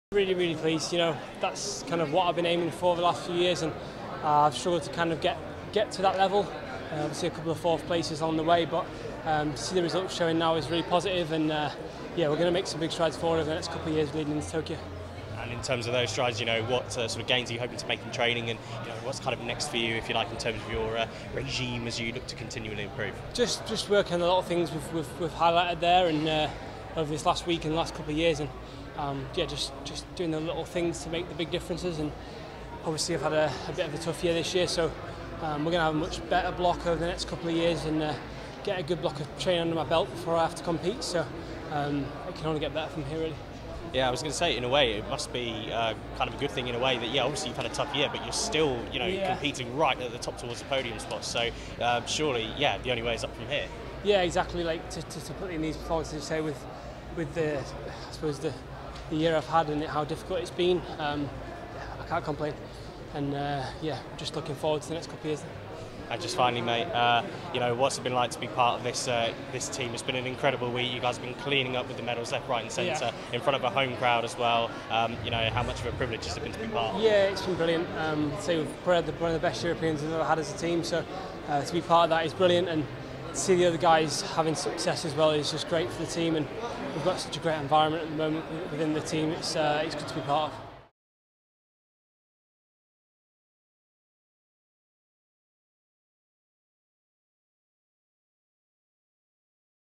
Max Litchfield earns another euro champs silver
Max Litchfield has made a positive return from injury and claimed his second European Championships silver of 2018 on the final night of racing at the Tollcross International Sports Centre.